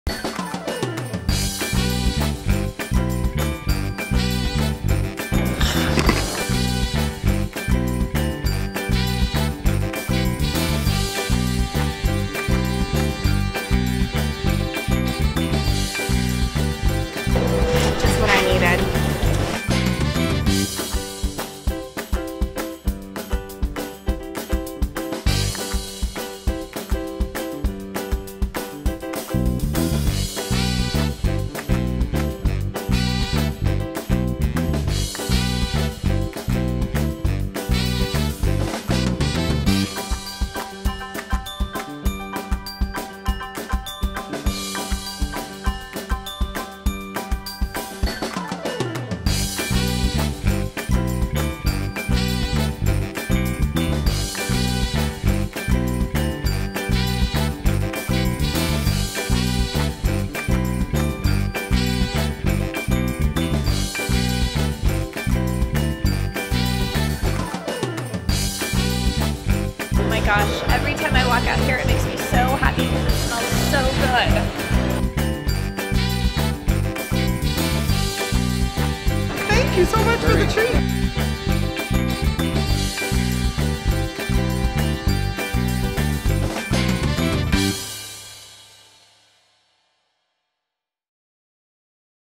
This Bar is Made Entirely Out of Gingerbread | Best Products
Head to LA to eat every single piece of this bar - it's entirely made of gingerbread.